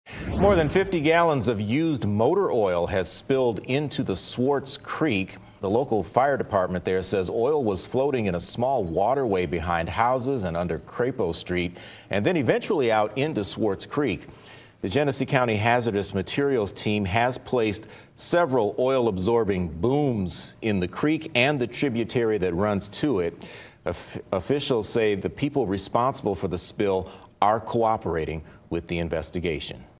Oils spills into the Swartz Creek
The Swartz Creek Area Fire Department says that oil has spilled into the creek.
Approximately 50 gallons of used motor oil was spilled, according to authorities.
Booms have been placed in the water to collect the spillage.